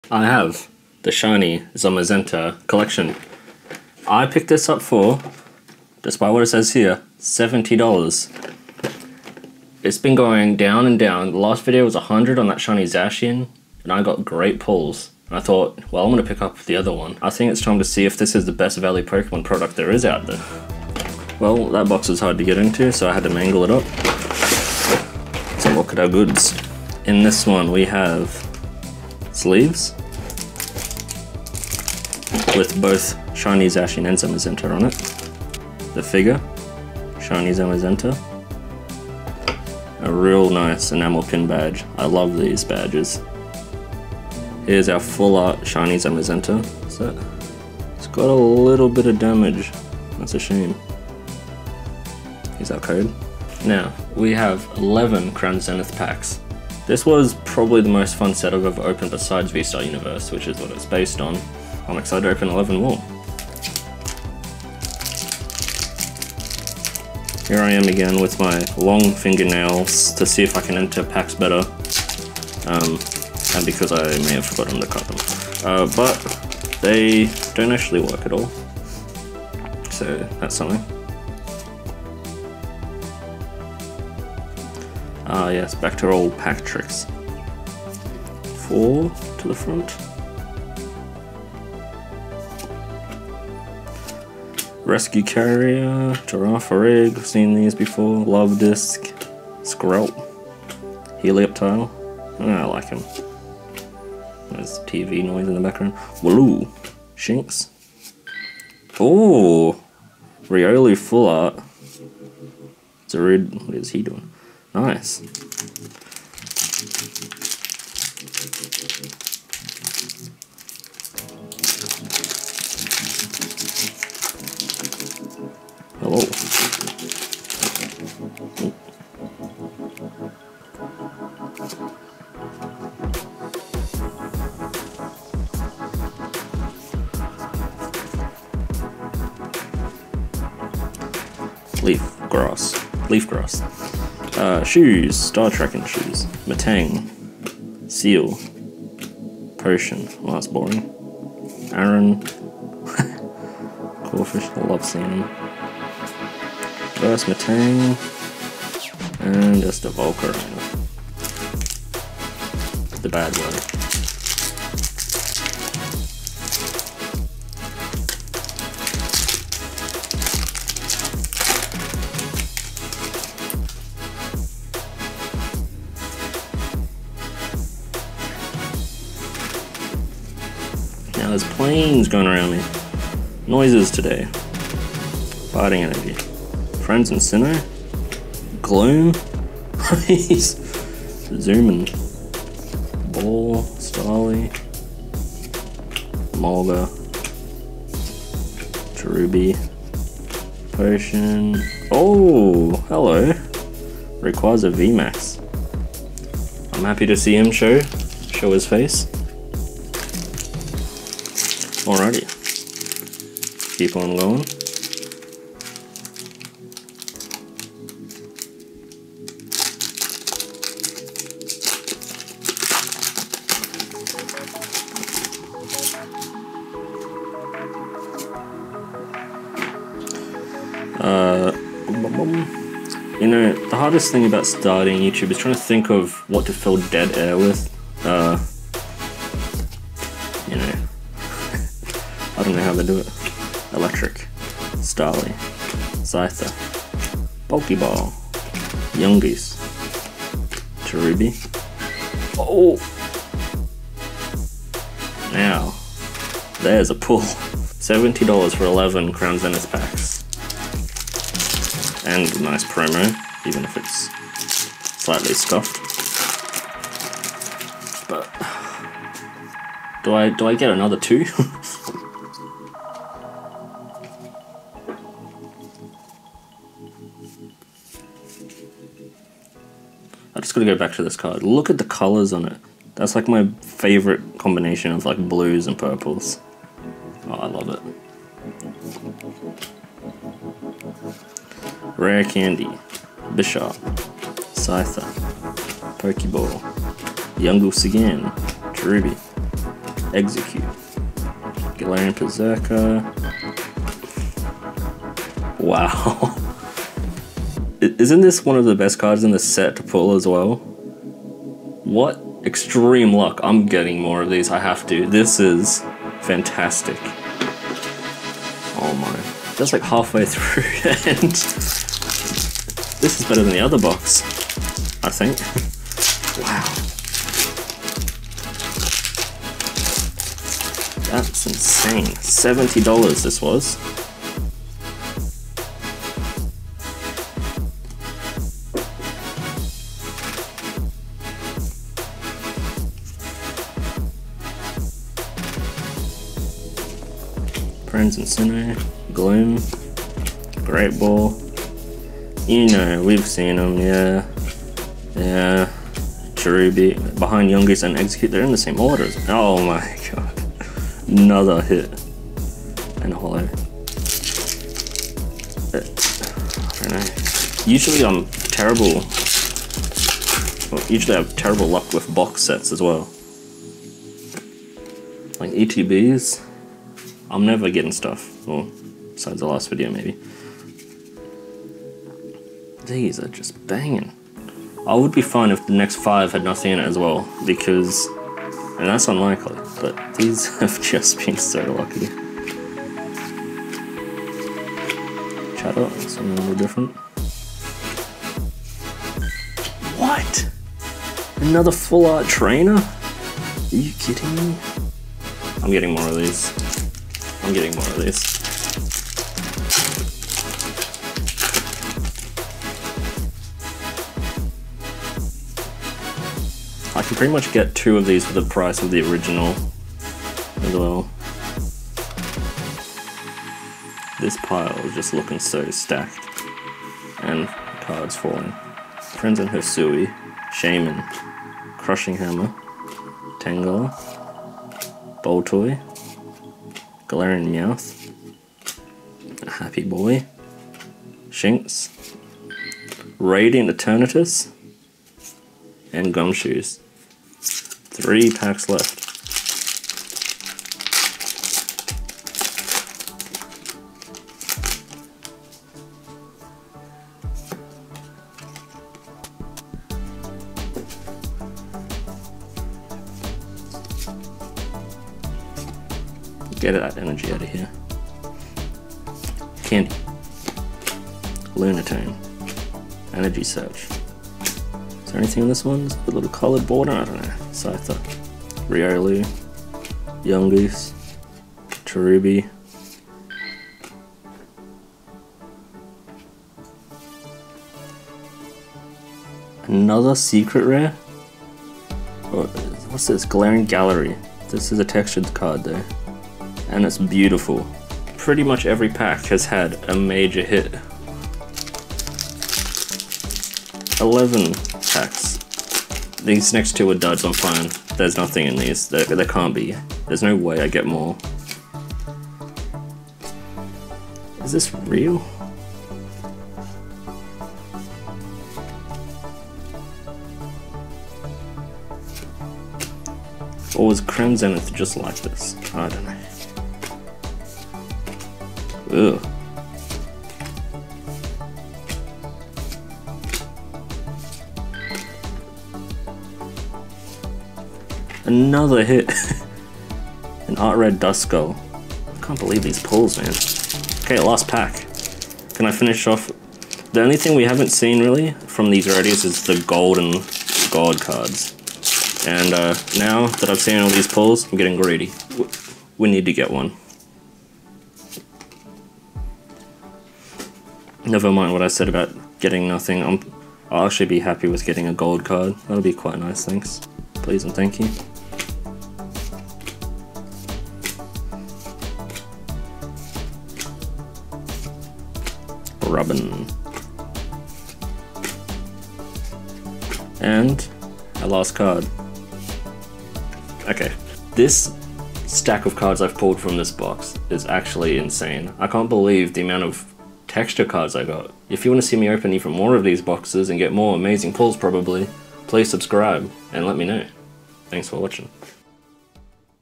Opening a LOADED Pokemon shiny Zamazenta box
After opening the shiny Zacian box I had to get the other, and somehow I got even better pulls!